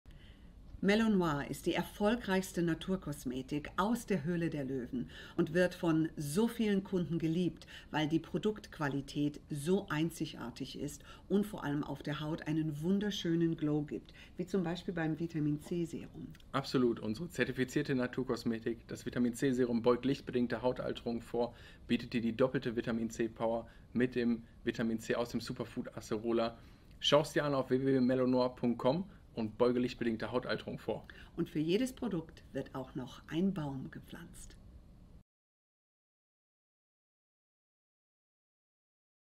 mellow NOIR - bekannt aus der TV Show "Die Höhle der Löwen"
Judith ist auch immer noch so verliebt wie wir in die zertifizierte Naturkosmetik von mellow NOIR! 🌿
Der Wachmacher für Deine Haut ☕
Jetzt im "Löwen-Deal" sichern und sparen!

Naturkosmetik aus Kaffee?
Ja, das funktioniert! Die wertvollen Fettsäuren und Antioxidantien im Kaffeeöl stärken nicht nur die natürliche Hautbarriere und helfen beim Speichern von Feuchtigkeit, sondern stimulieren auch die Kollagensynthese und tragen zum Schutz des Kollagens bei. So kann unser Kaffeeöl Deiner Haut neue Spannkraft verleihen und unterstützt sie zusätzlich bei der schnelleren Regeneration. Ein echter Wachmacher für Deine Haut!

Unser Vitamin C Serum beugt mit der doppelten Vitamin C Power lichtbedingter Hautalterung vor. In besonders pflegender Formulierung mit über 1,5% natürlichem Vitamin C.

🌿  vegane, natürliche Inhaltsstoffe
🌍. klimaneutral & nachhaltig
🌳  1 Produkt = 1 Baum
🦁  Bekannt aus der TV-Show "Die Höhle der Löwen"

Überzeuge Dich selbst von der Kraft des Kaffeeöls und teste jetzt unsere zertifizierte Naturkosmetik